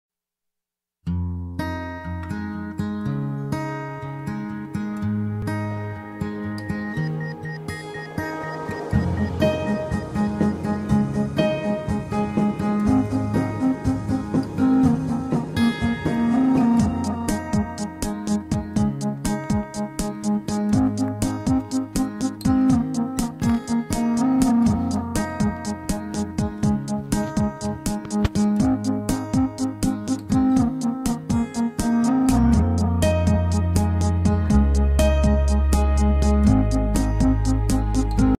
Help me identify this ant species
I found this queen ant in a forest outside Stockholm, Sweden.